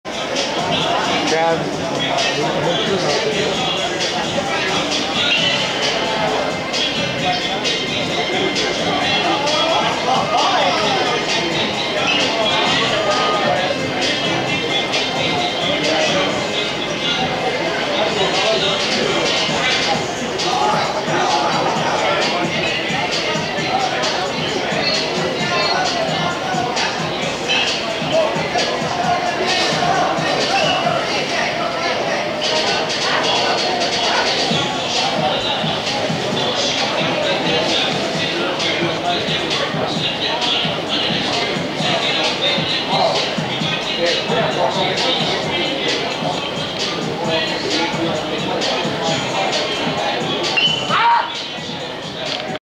Rain delay at NJCAA Region XI tournament (5-7-15)
NIACC baseball coach Travis Hergert and others help get the field ready during a rain delay in the Iowa Central/Ellsworth game at the NJCAA Region XI baseball tournament on Thursday at Riverfront Stadium in Waterloo.